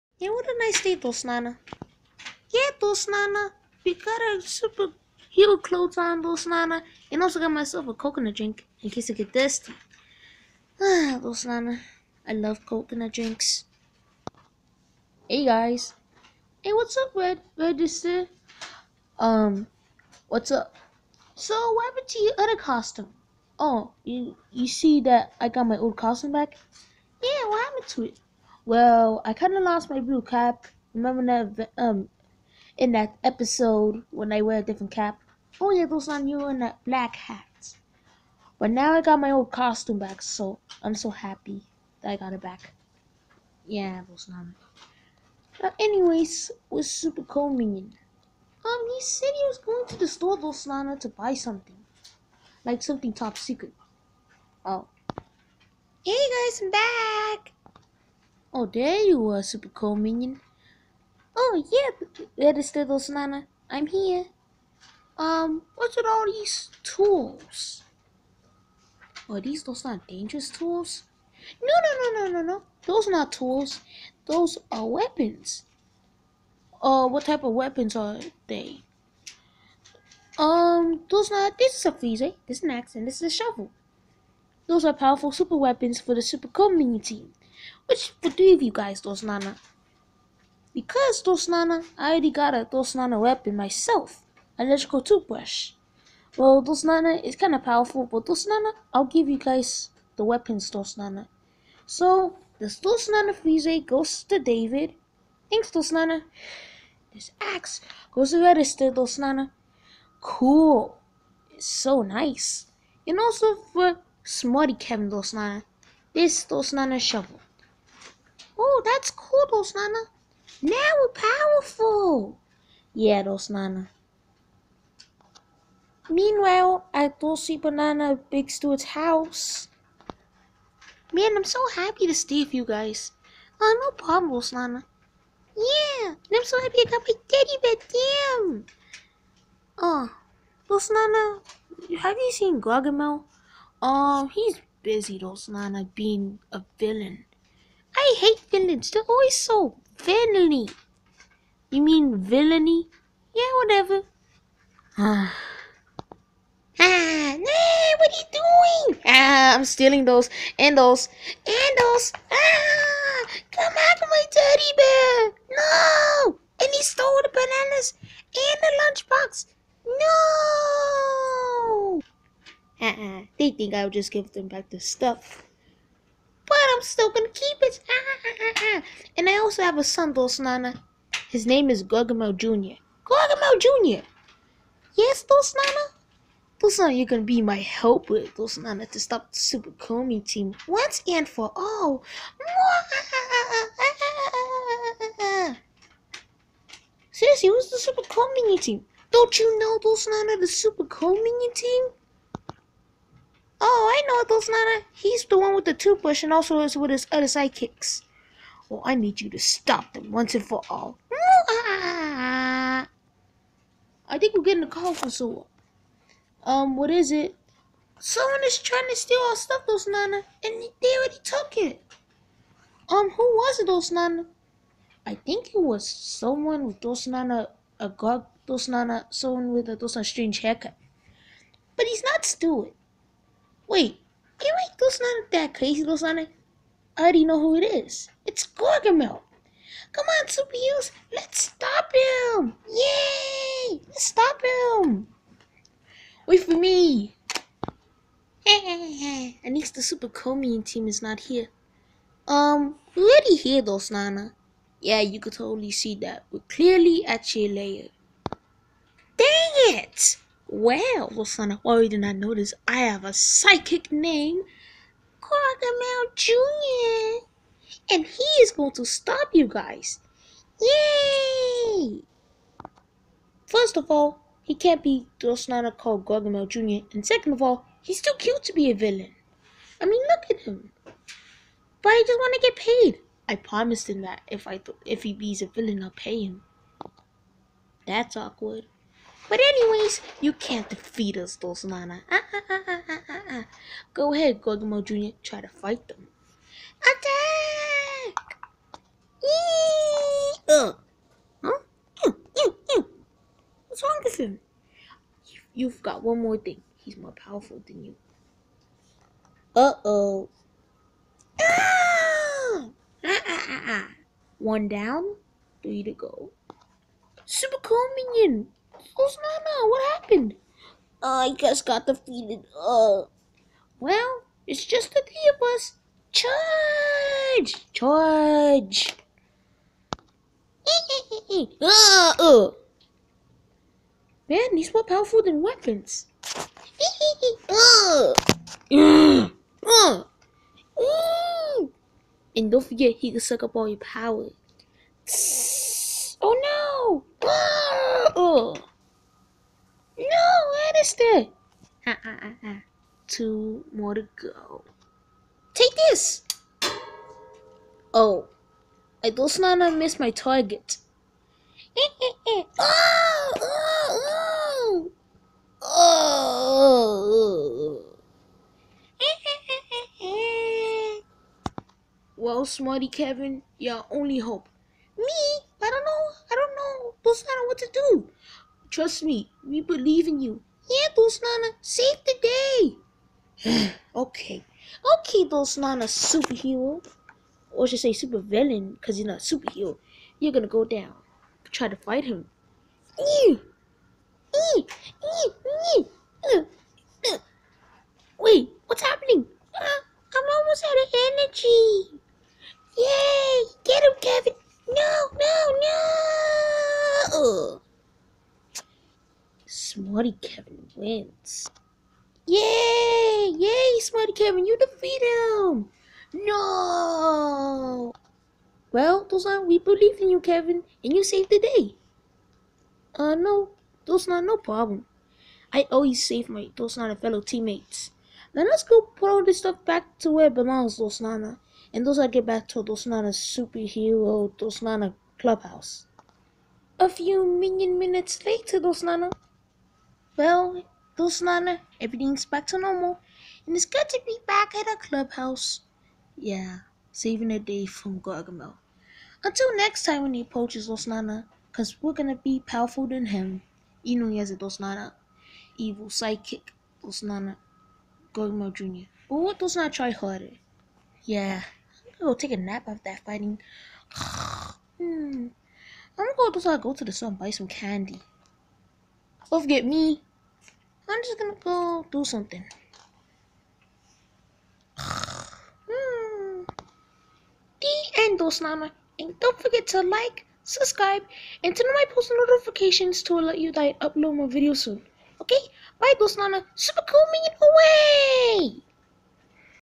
Super Cro Minion Series Episode 3: GargaMel's Sidekick
This is about when GargaMel has a sidekick named GargaMel Jr which is going to stop the Super Cro Minion Team. But, The Super Cro Minion Team defeated GargaMel's sidekick and saved the day.